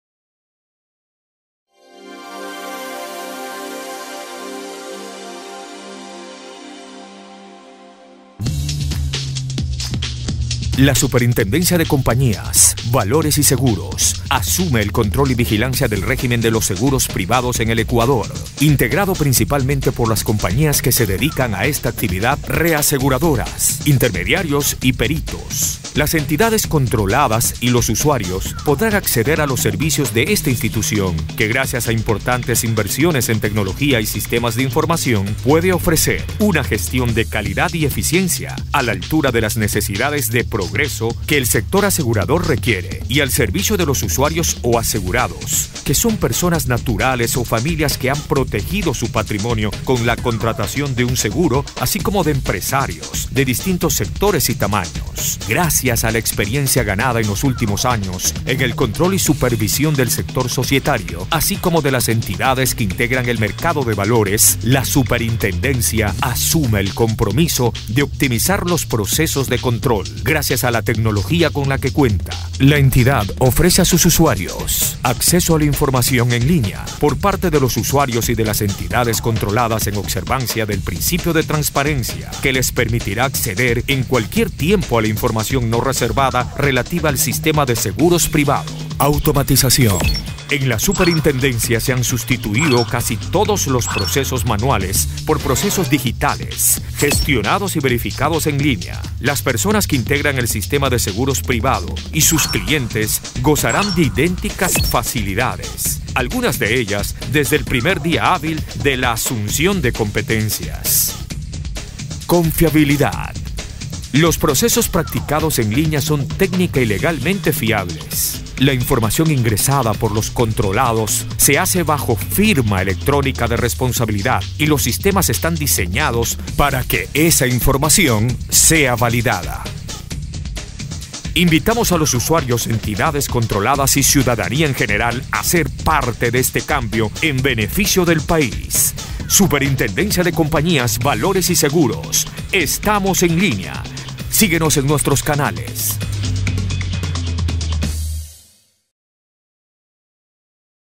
El control de los seguros cambió de casa
La Superintendencia de Compañías y Valores, asumió el control y vigilancia de todo el régimen de seguros privados en el Ecuador, integrado principalmente por las empresas de seguros, reaseguradoras, intermediarios y peritos.